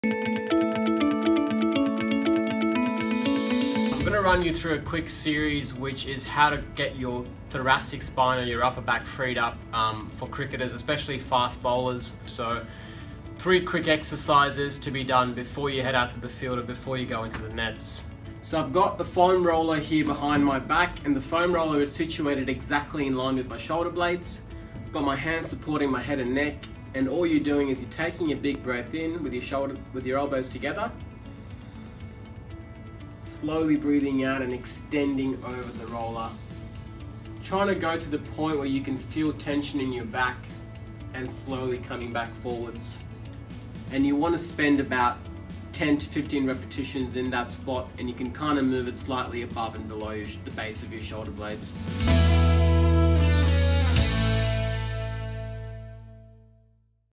Thoracic Mobility Part 1 (Start with Extension)
In this video, Adrian starts with a simple thoracic extension. This gets his thoracic spine comfortable with getting back into neutral and even into extension (think the opposite of sitting flexed at the computer).

This opens up his chest and shoulder and also starts to reverse some of the curves and stiffening around his middle spine. This alone can help reduce neck, shoulder and lower back niggles.